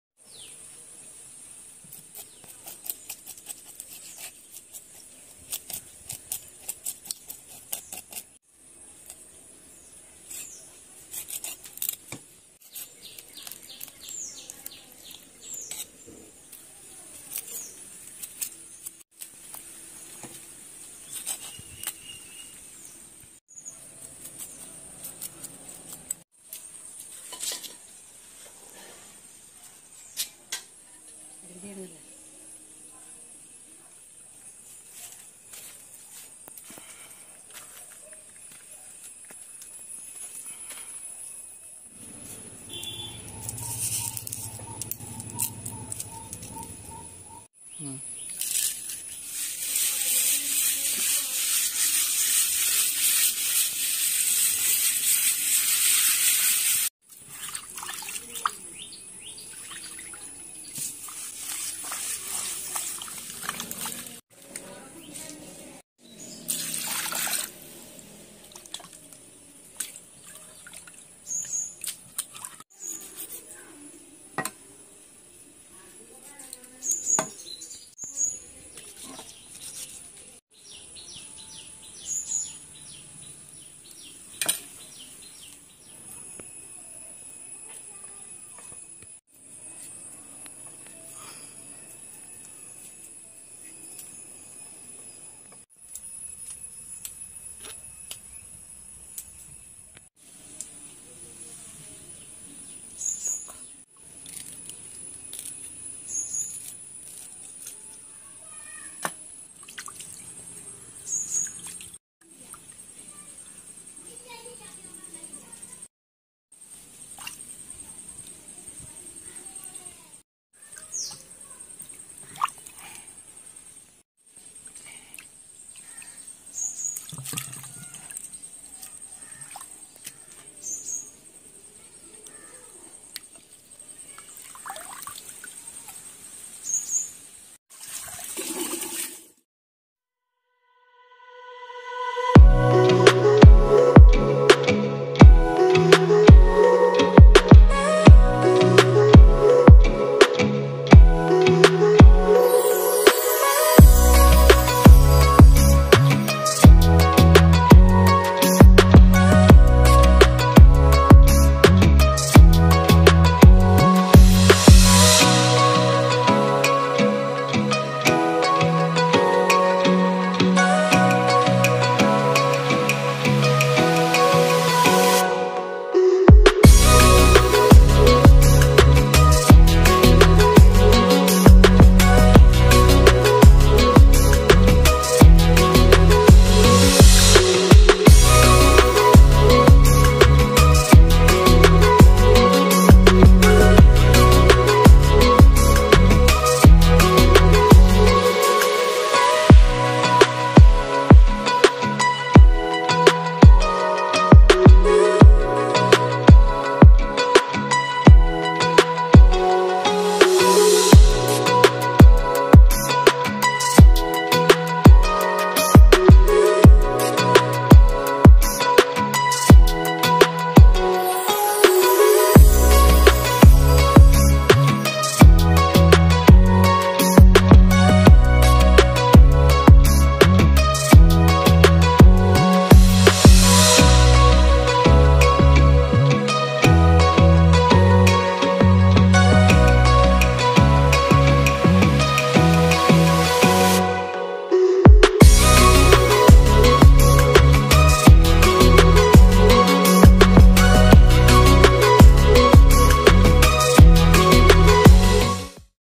Sardine Fish Curry || Challa or Mathi Curry || Kerala style
Sardine Fish Curry
.very healthy and tasty dish
. Kerala style
easy to make
 tasty and easy to make.
A must try dish.
.Watch till the End.

.BGM